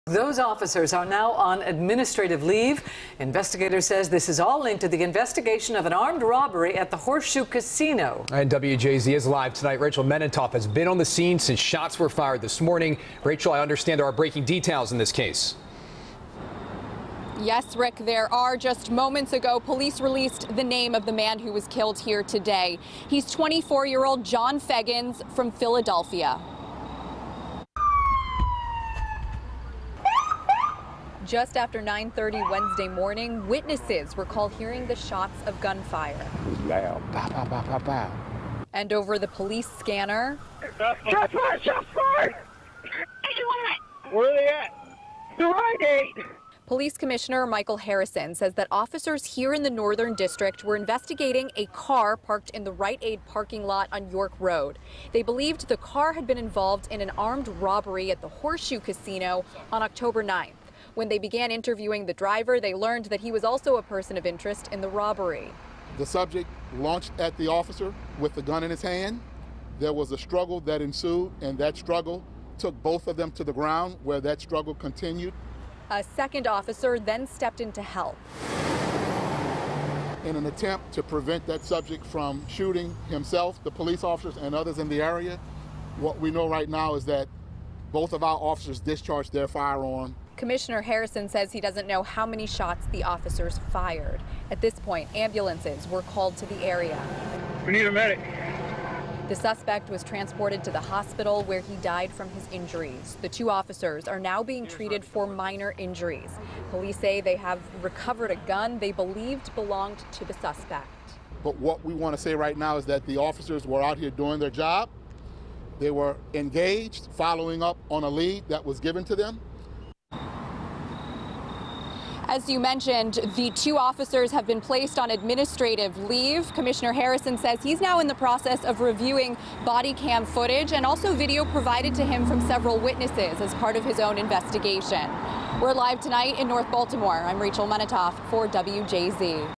John Feggins Identified As Suspect Killed In Officer-Involved Shooting At Baltimore Rite Aid
An armed suspect was shot and killed and two officers were injured during a police-involved shooting at a Rite Aid in north Baltimore Wednesday morning.